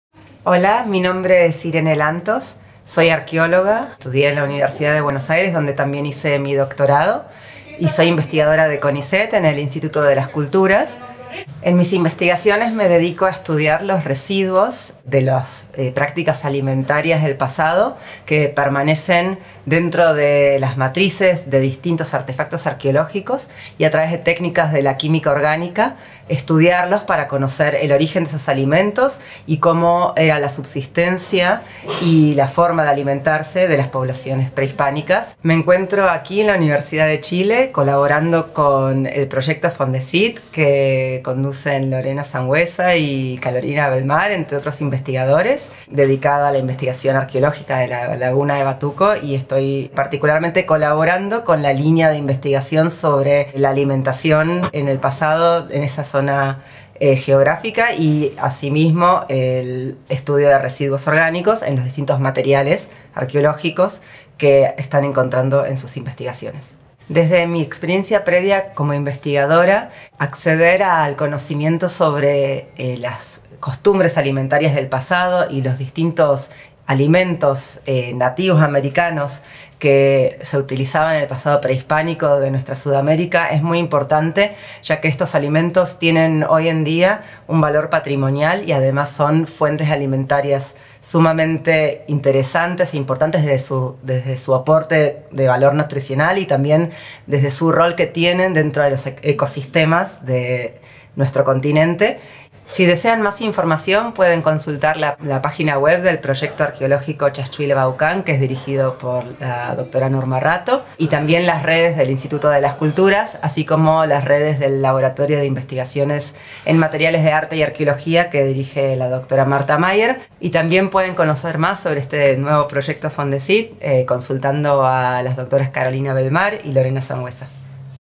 Irene Lantos. Arqueología y Alimentación Prehispánica
Seminario realizado en el marco del proyecto Fondecyt 1220203 "Ocupación prehispana en torno al Totoral. Patrón de asentamiento y uso de recursos en la Laguna de Batuco". Investigadora responsable: Lorena Sanhueza. Coinvestigadores: Carolina Belmar, Fernanda Falabella, Itaci Correa, Luis Cornejo y Antonio Maldonado.